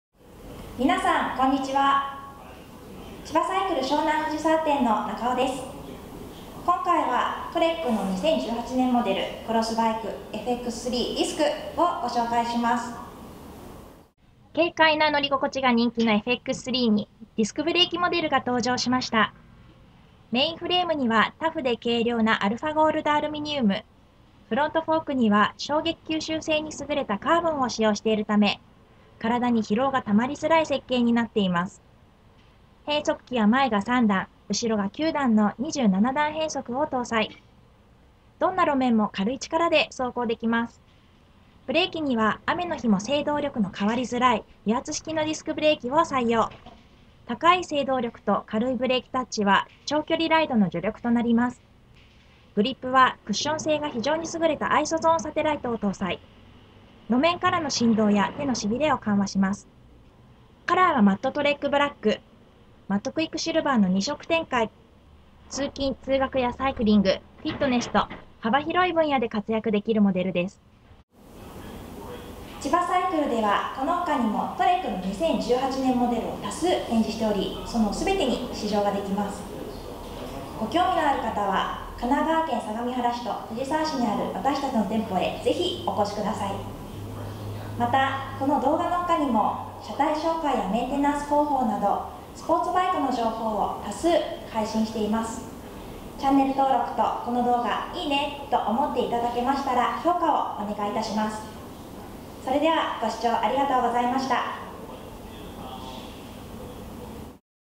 TREK FX3 Disc(トレック FX3 ディスク) 2018年モデル レビュー
ちばサイクル 湘南藤沢店スタッフ・中尾(なかお)がTREK 2018年モデルのクロスバイク・FX3 Discをご紹介！

軽量アルミフレームをベースにフロントフォークにはカーボンを使用。これにより軽量化はもちろんのこと、路面から伝わる振動も緩和されるので長距離走行時の腕の疲れが激減します。また、クッション内装ハンドル「IsoZoneバー」と、その専用グリップが腕への負担をさらに軽減してくれます。油圧ディスクブレーキを搭載しているのでどんな天候下でも信頼できる制動力をもたらします。

↓こちらから、より詳細なスペックを確認できます。↓
FX3 Disc